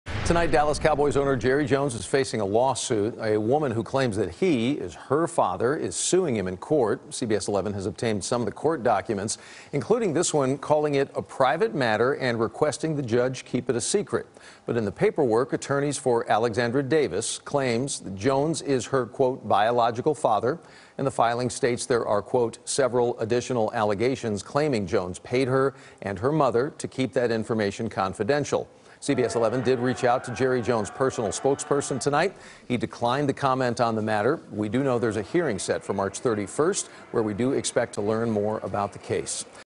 Woman Sues Cowboys Owner Jerry Jones Claiming He Is Her Biological Father
The records and details of the case have been sealed. Jim Wilkinson, the personal spokesperson for Jerry Jones, declined to comment.